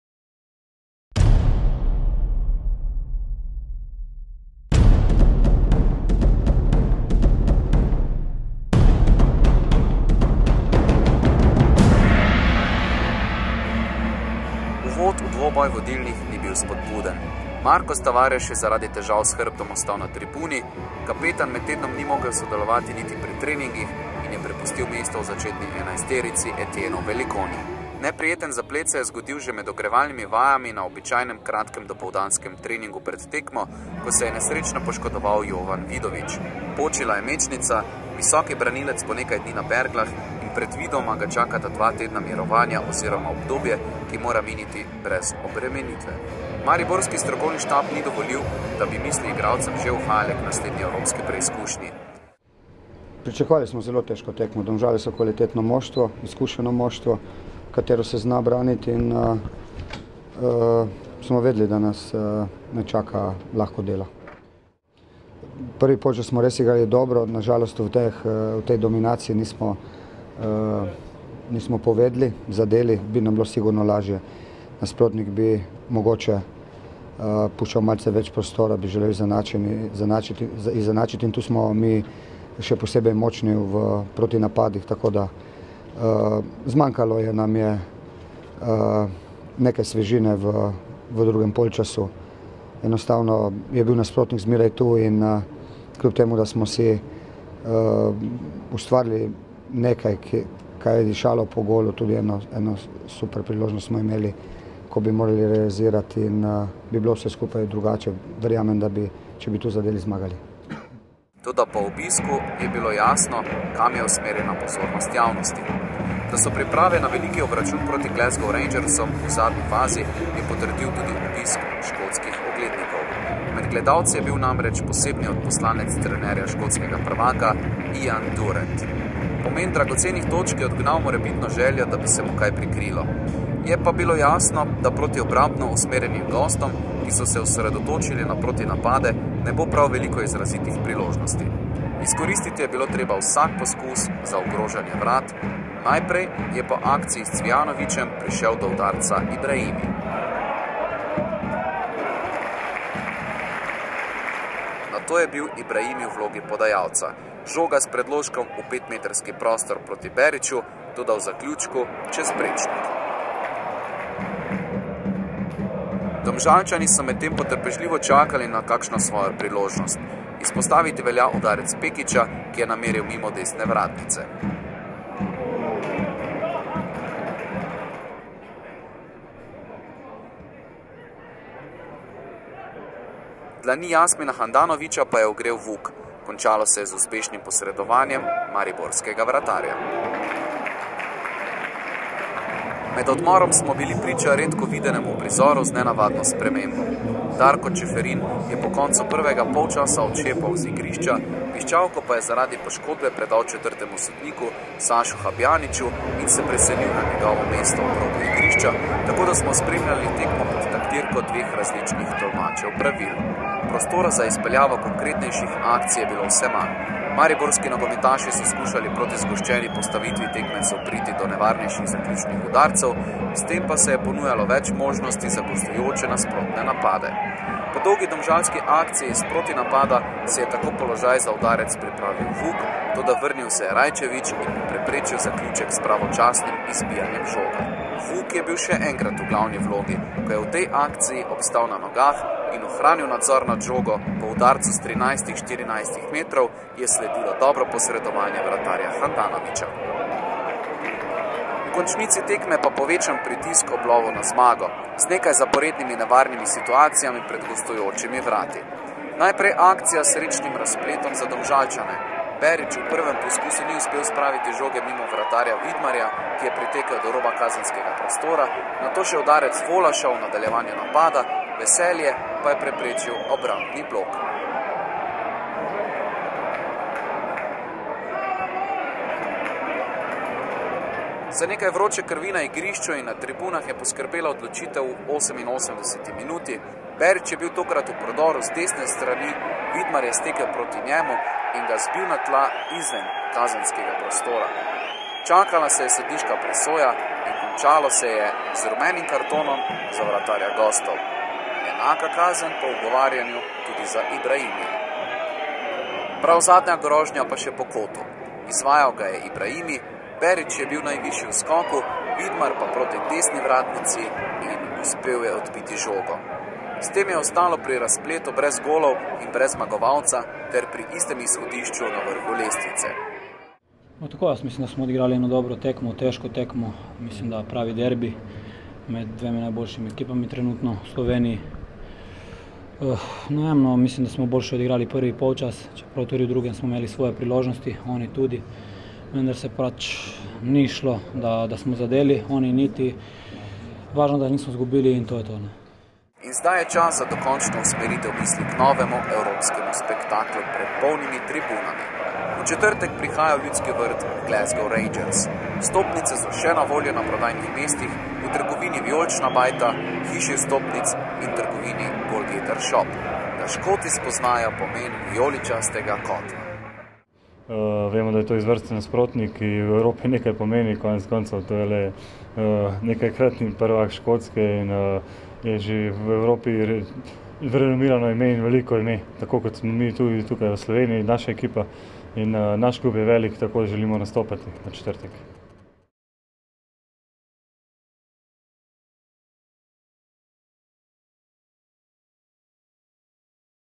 NK Maribor - NK Domžale
SUPERPOKAL SLOVENIJE 2011 - Maribor, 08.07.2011 Ljudski vrt. NK MARIBOR - NK DOMŽALE 1:2 (0:1)